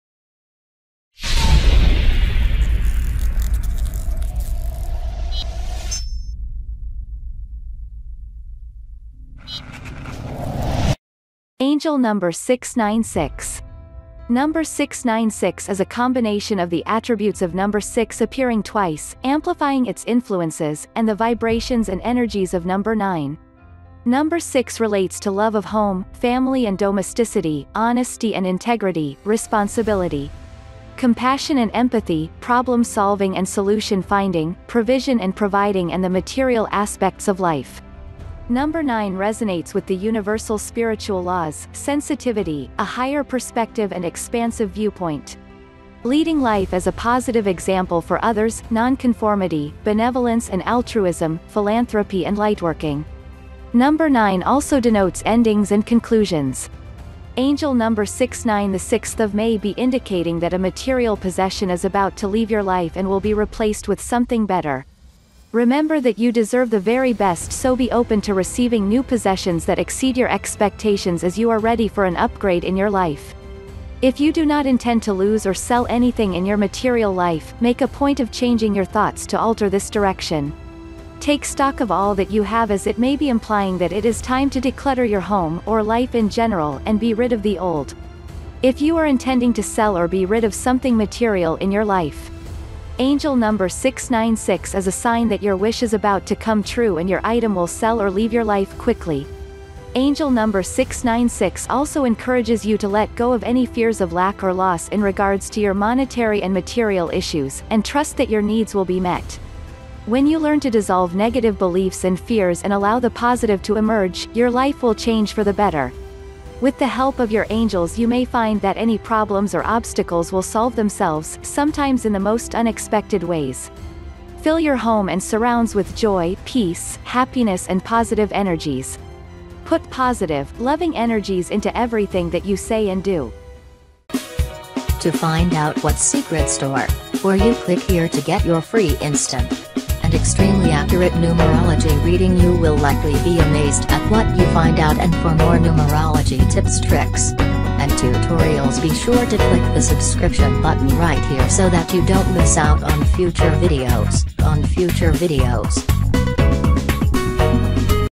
🔴 Angel Number Meanings 696 ✅ Seeing 696 ✅ Numerology Box
Number 696_meaning is a combination of the attributes of number 6 appearing twice, amplifying its influences, and the vibrations and energies of number 9. Number 6 relates to love of home, family and domesticity, honesty and integrity, responsibility, compassion and empathy, problem-solving and solution-finding, provision and providing and the material aspects of life. Number 9 resonates with the Universal Spiritual Laws, sensitivity, a higher perspective and expansive viewpoint, leading life as a positive example for others, non-conformity, benevolence and altruism, philanthropy and lightworking. Number 9 also denotes endings and conclusions.

Angel Number 696 may be indicating that a material possession is about to leave your life and will be replaced with something better. Remember that you deserve the very best so be open to receiving new possessions that exceed your expectations as you are ready for an upgrade in your life.

If you do not intend to lose or sell anything in your material life, make a point of changing your thoughts to alter this direction. Take stock of all that you have as it may be implying that it is time to de-clutter your home (or life in general) and be rid of the old. If you are intending to sell or be rid of something material in your life, Angel Number 696 is a sign that your wish is about to come true and your item will sell or leave your life quickly.

angel_number_696 also encourages you to let go of any fears of lack or loss in regards to your monetary and material issues, and trust that your needs will be met. When you learn to dissolve negative beliefs and fears and allow the positive to emerge, your life will change for the better. With the help of your angels you may find that any problems or obstacles will solve themselves - sometimes in the most unexpected ways.

Fill your home and surrounds with joy, peace, happiness and positive energies. Put positive, loving energies into everything that you say and do.